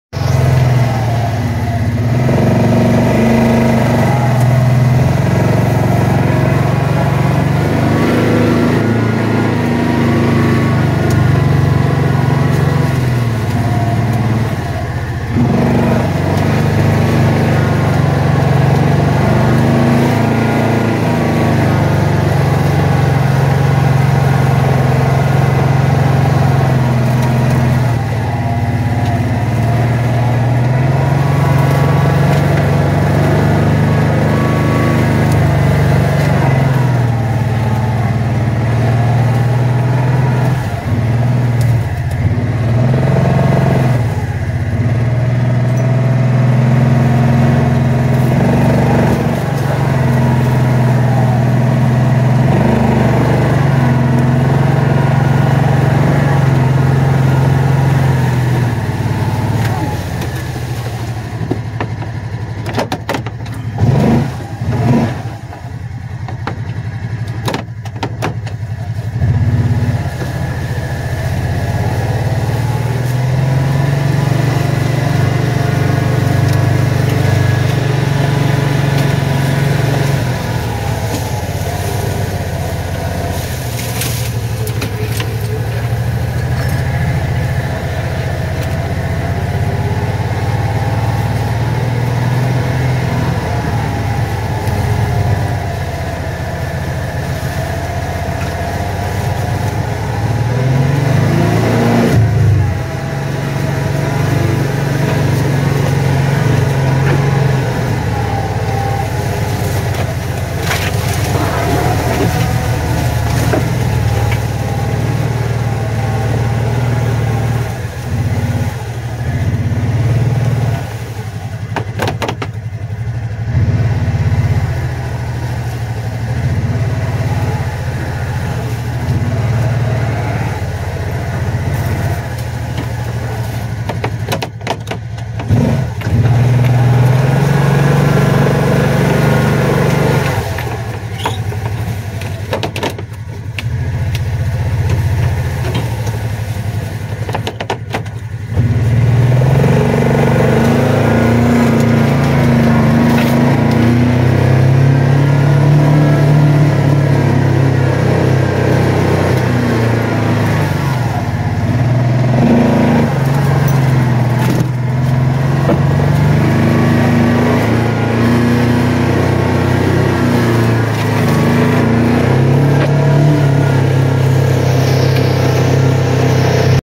Off Roading/Kawasaki Teryx4 LE 4X4 Side By Side Drive Through Our Woods
A fun drive on our nature trail up the hill in our woods in our side by side.